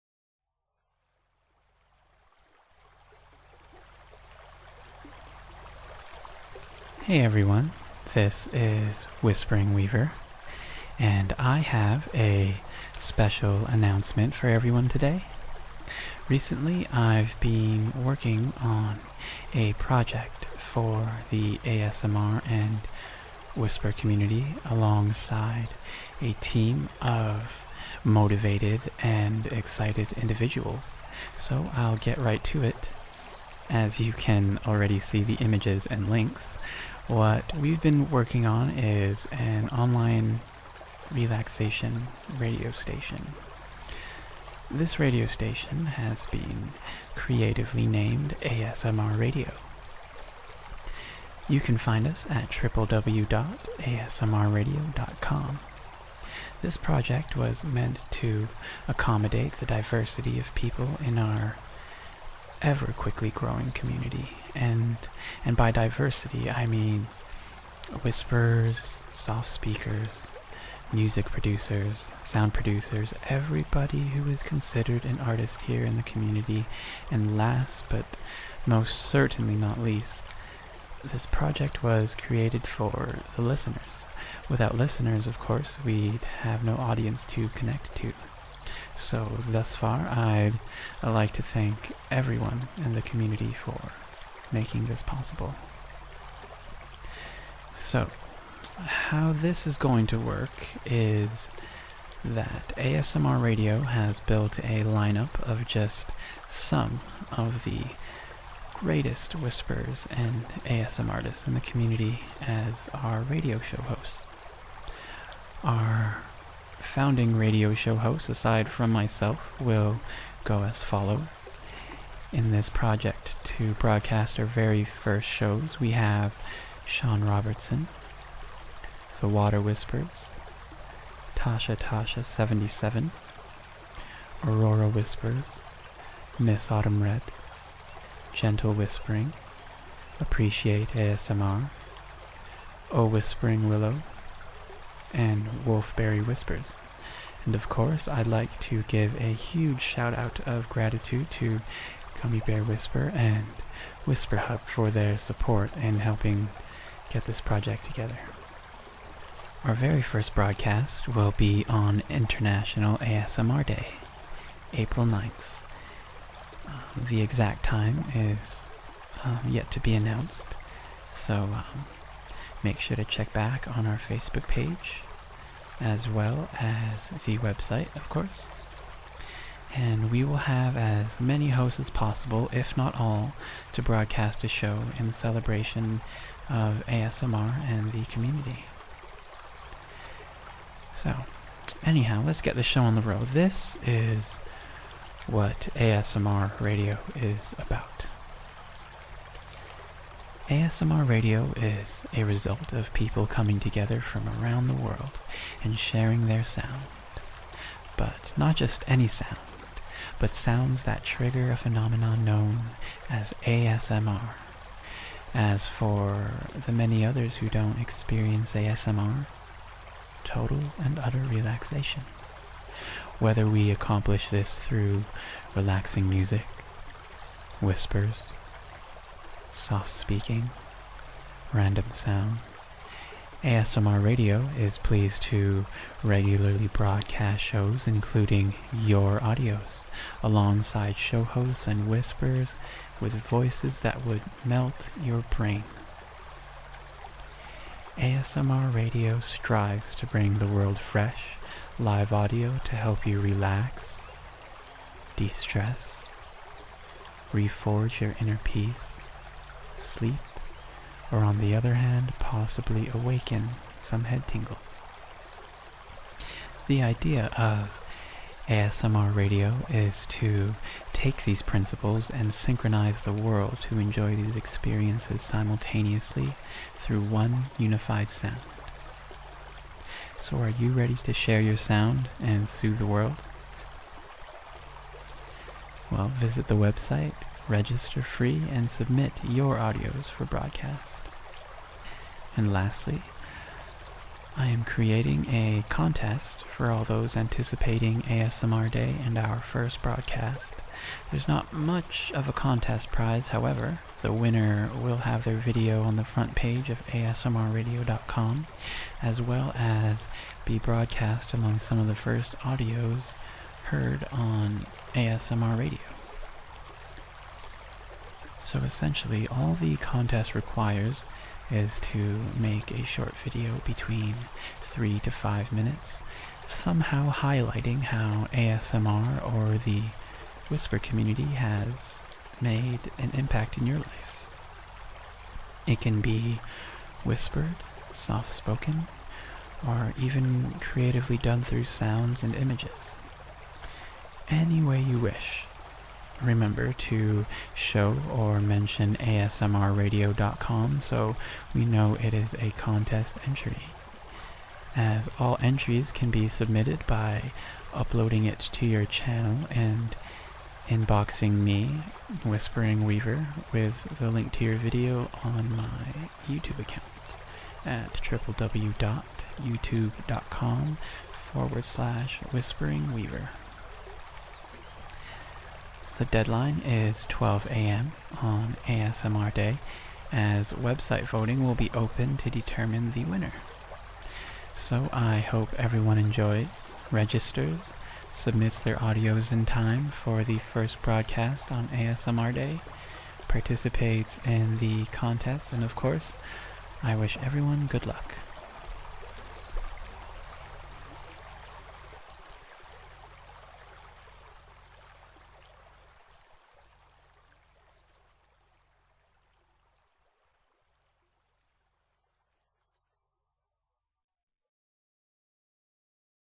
#7 ASMR [Soft Spoken] ASMR Radio Unveils + Contest
[I've changed the contest details. Extended the video time limit. It is now 5-15 minutes instead of 3-5 minutes. and participate!]

Hey everyone, this is WhisperingWeaver. And I have a special announcement for every one today. Recently I've been working on a project for the ASMR and whisper community, alongside a team of motivated, and excited individuals. So I'll get right to it, as you can already see the images and links. What we've been working on is an online relaxation radio station.

This radio station, has been creatively named ASMR Radio. This project was meant to accommodate the diversity of people in our ever quickly growing community. And by diversity, I mean whisperers, soft-speakers, music producers, sound producers - everybody who is considered an artist here in the community and last but most certainly not least, this project was created for the listeners! Without listeners of course we'd have no audience to connect to. So thus far, I'd like to thank everyone in the community for making this possible.

So how this is going to work is that ASMR Radio has built a line up of just some of the greatest whisperers and ASMRtists in the community as our Radio Show hosts.

Our founding radio show hosts aside from myself will go as follows. In this project to broadcast our very first shows, we have...

Shaun Robertson
TheWaterwhispers
TashaTasha77
AuroraWhispers
MsAutumnRed
GentleWhispering
AppreciateASMR
oWhisperingWillow
WolfBerryWhispers

and of course I'd like to give a huge shoutout of gratitude to GummybearWhisper and Whisperhub for their support in helping get this project together.

Our very first broadcast will be on Intl ASMR day. And we will have as many hosts as possible, if not all, to broadcast a show in celebration of ASMR and the community.
Let's get this show on the road. This is what ASMR Radio is about.

ASMR Radio is a result of people coming together from around the world and sharing their sound. Not just any sound, but sounds that trigger a phenomenon known as ASMR. As for the many others who don't experience ASMR - total and utter relaxation.
Whether we accomplish this through relaxing music, whispers, soft speaking, random sounds, ASMR Radio is pleased to regularly broadcast shows including *your* audios, alongside show hosts and whisperers with voices that would melt your brain!
ASMR Radio strives to bring the world fresh, live audio to help you relax, de-stress, reforge your inner peace, sleep or on the other hand, possibly awaken some head tingles!

The idea of ASMR Radio is to take these principles and synchronize the world to enjoy these experiences simultaneously through one unified sound.
Are you ready to share your sound and soothe the world?

And lastly, I am creating a contest for all those anticipating ASMR day and our first broadcast. There's not much of a contest prize, however, the winner will have their video on the front page of ASMRradio.com as well as be broadcast among some of the first audios heard on ASMR Radio.

All the contest requires is to make a short video, between 3-5 minutes somehow highlighting how ASMR or the whisper community has made a big impact in your life. It can be whispered, softspoken, or even creatively done through sounds. Any way you wish. Remember to show or mention ASMRradio.com so we know it is a contest entry. All entries can be submitted by uploading it to your channel and inboxing me with the link to your video on my YouTube account
The deadline is 12AM on ASMR day as website voting will be open to determine the winner.

So I hope everyone enjoys, registers, submits their audios in time for the first broadcast on ASMR day, participates in the contest and of course...I wish everyone good luck!